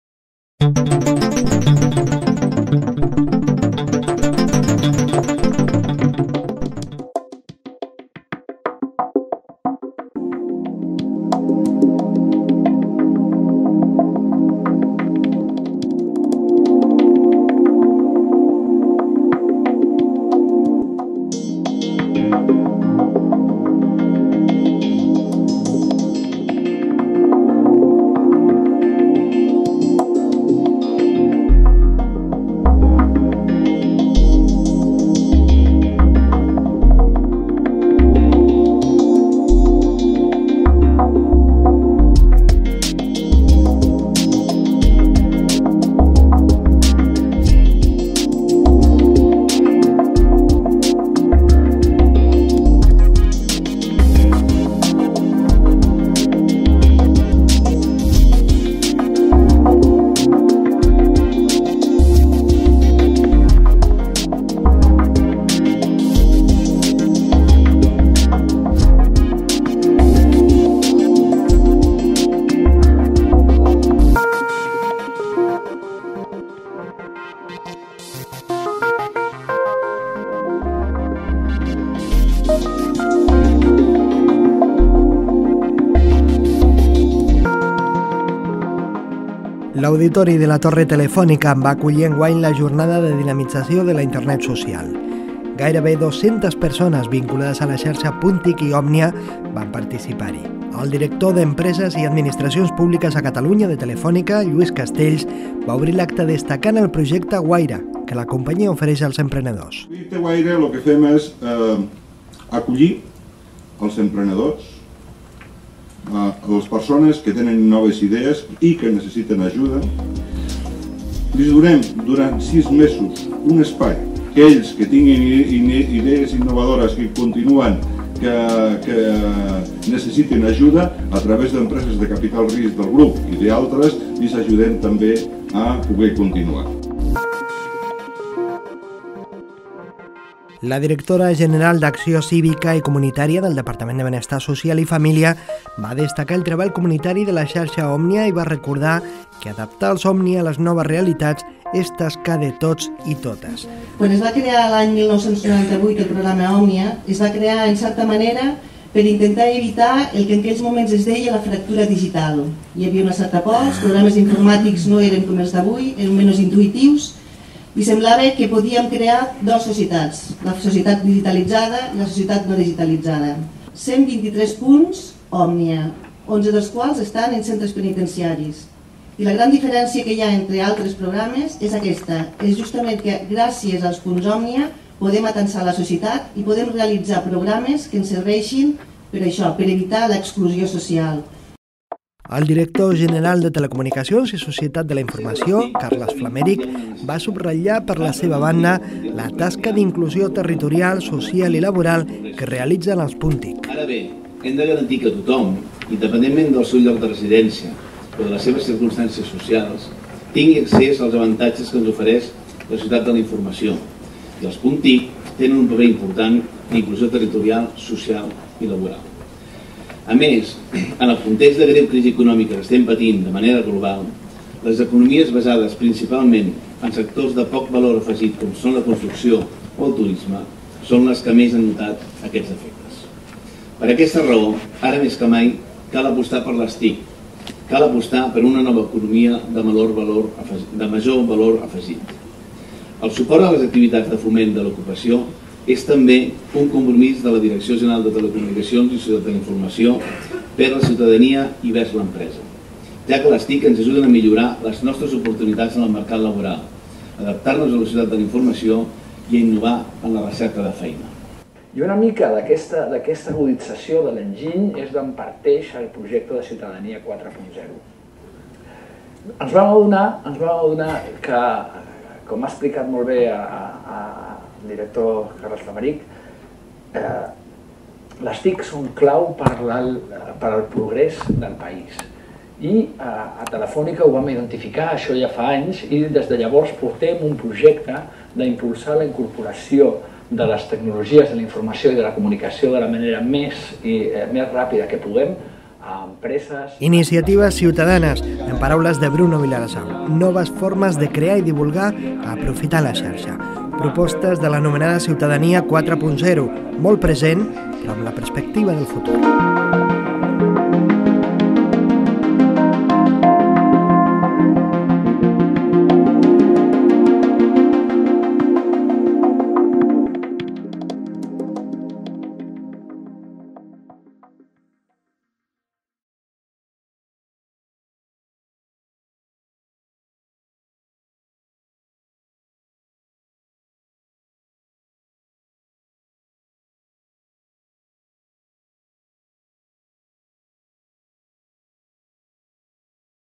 Vídeo resum de la Jornada de dinamització de la Internet Social (1/4)
L'auditori de la torre telefònica va acollir enguany la Jornada de dinamització de la Internet Social. En aquest primer vídeo, podeu veure els detalls de l'acte d'inauguració de la jornada i uns moments de la presentació sobre Ciutadania 4.0